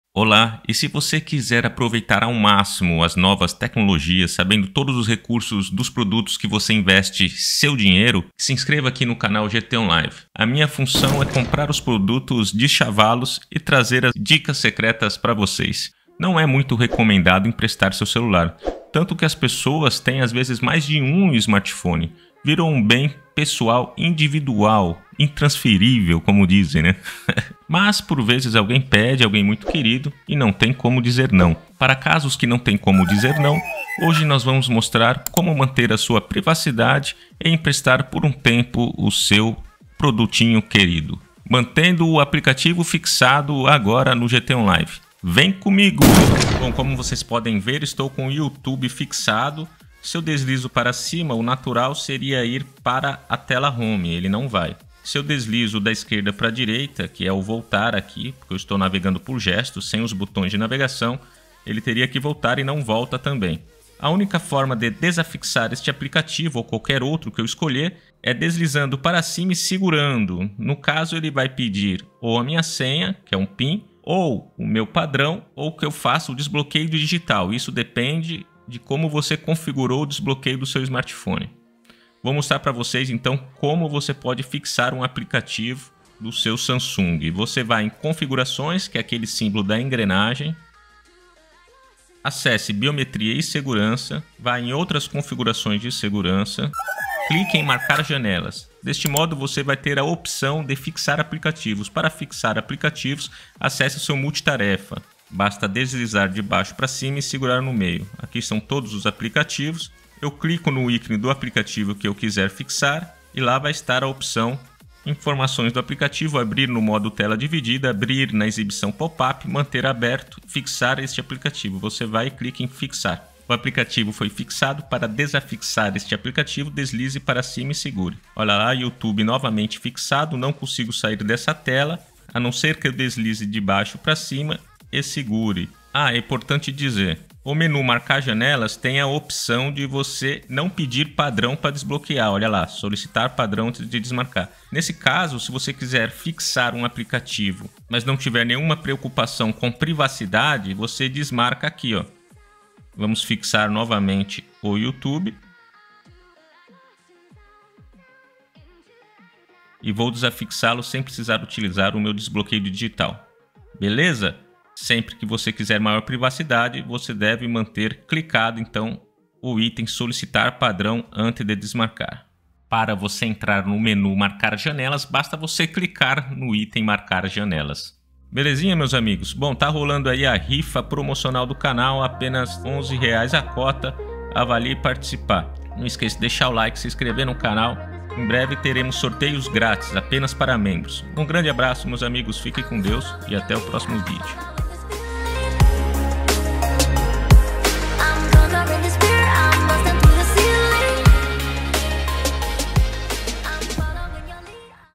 💥Dicas e Truques! Como fixar APP na Tela de seu Samsung Galaxy (One Ui 4.1)
Antes de emprestar seu celular ou smartphone para qualquer pessoa, evite imprevistos e tranque a tela do seu Samsung. Neste vídeo: Dicas Samsung Galaxy One Ui 4.1 (segurança e privacidade).

👉🏾Compre na Magazine Luísa por meio da LOJA GT ONLIVE.
👉🏾GRUPOS GT ONLIVES INFORMAÇÕES GRATUITAS. FAÇA PARTE DESSA FAMÍLIA!